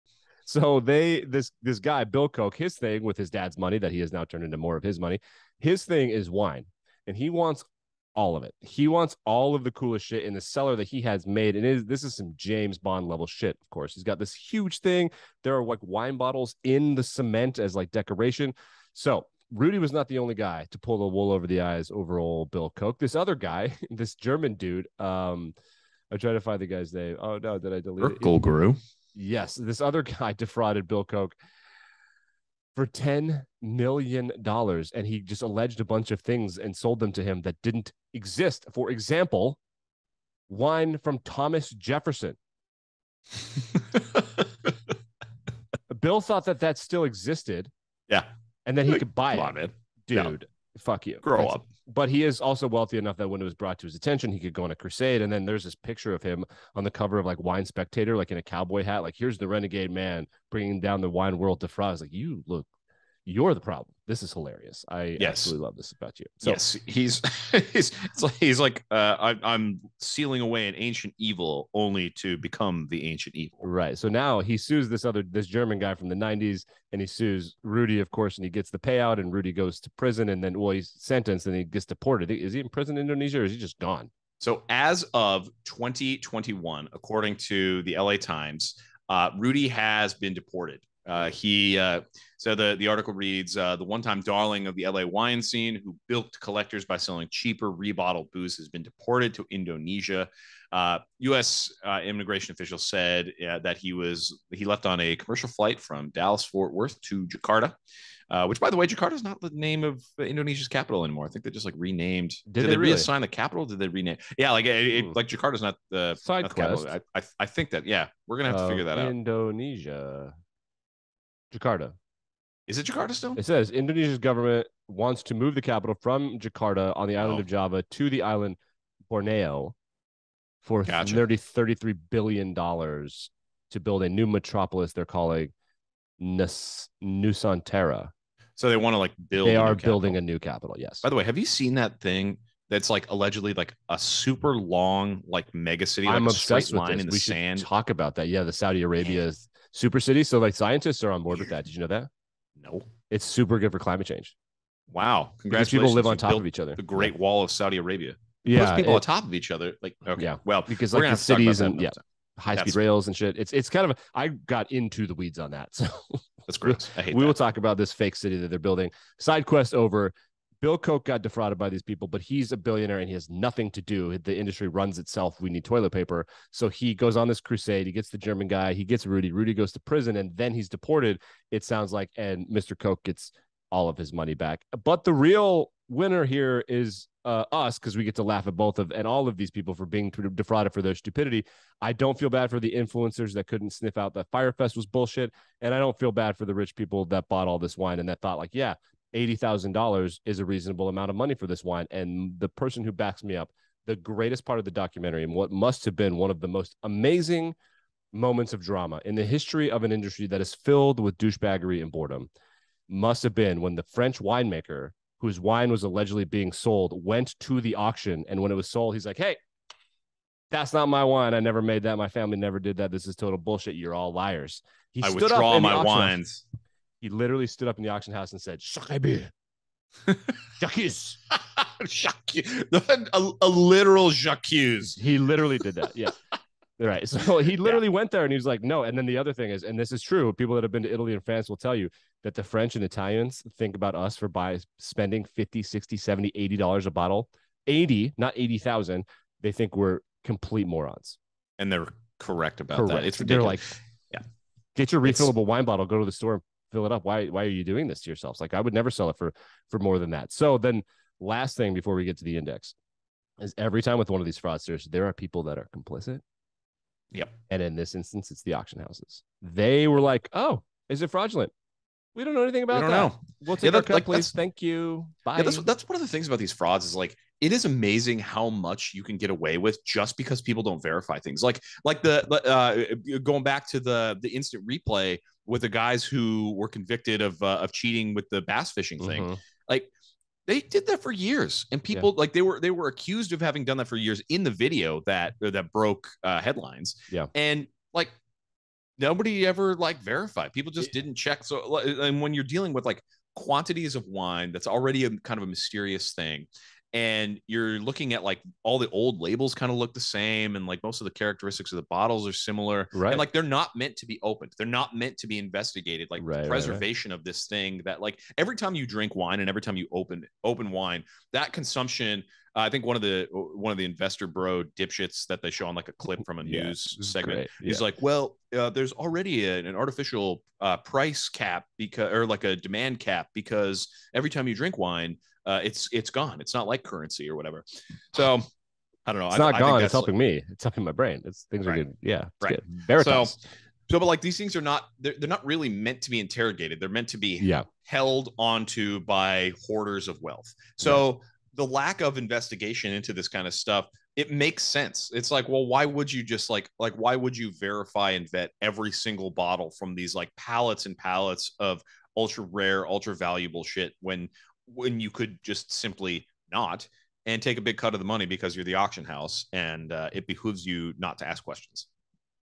Bill Koch Ripped Off by Super Tasting Wine Genus Rudy Kurniawan and Why We Do Not Feel Bad for Bill
Listen to Episode 44 of Game Theory

How Bill Koch and other wine "intelligencia" were ripped off by a super-tasting Savant. And why we don't feel bad.

Sources:

Googliography:
Sour Grapes Documentary